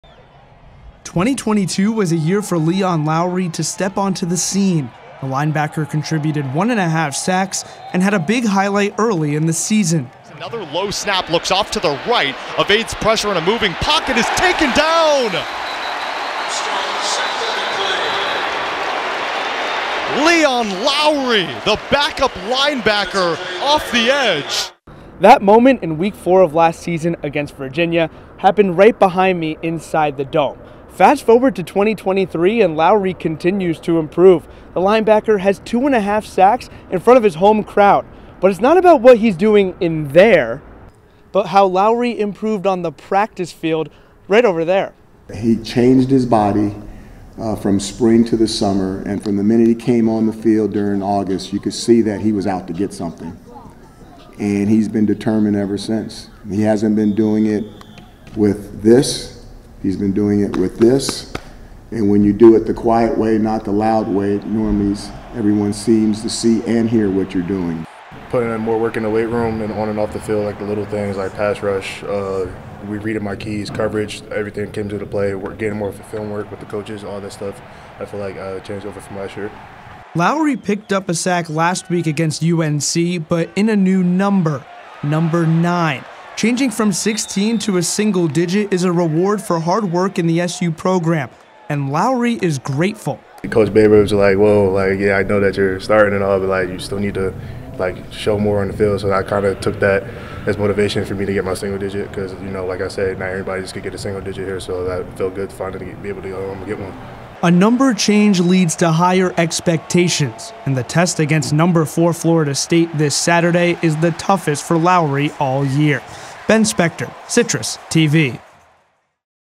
Leon Lowery Continues To Improve
SU linebacker Leon Lowery has continued to show improvement this season. The redshirt sophomore has even been recognized this year for his hard work. Ben Spector details how Lowery has made a leap from 2022 to 2023.